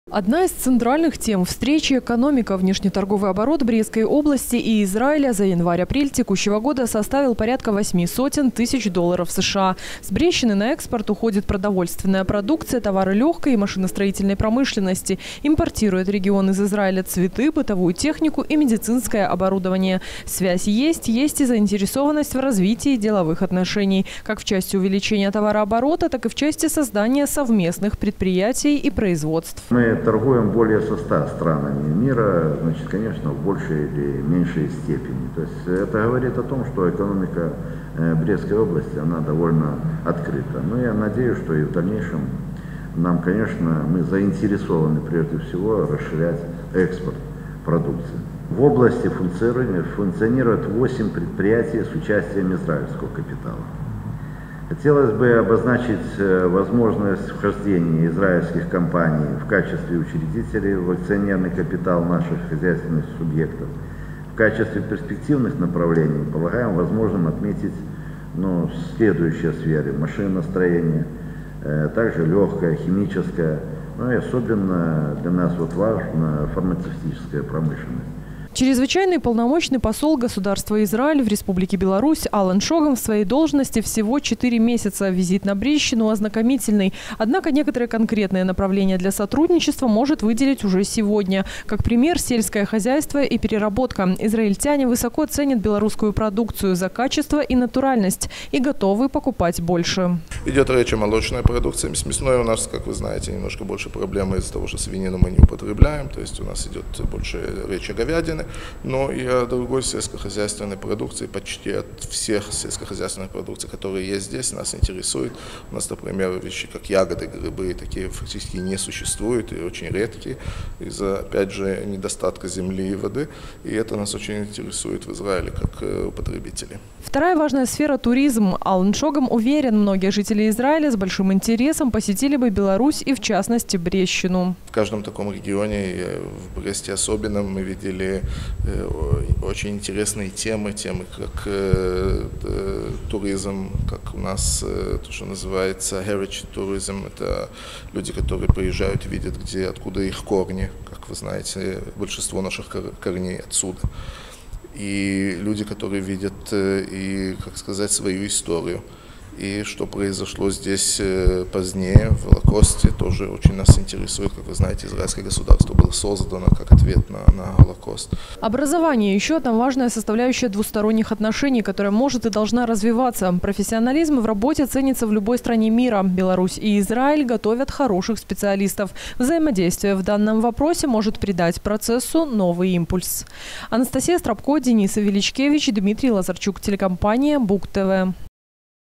2017-06-20 г. Брест. Визит Посла Израиля в Беларуси на Брестчину. Новости на Буг-ТВ.
Журналист А. Страпко Оператор Д. Величкевич
Чрезвычайный и Полномочный Посол Государства Израиль в Республике Беларусь Алон Шогам с официальным визитом находится на Брестчине. В программе – посещение предприятий, организаций и памятных мест региона, а также встреча с руководством края. В ходе последней были подняты вопросы сотрудничества сегодня и перспективы на будущее. Побывала на мероприятии и съёмочная группа нашего телеканала.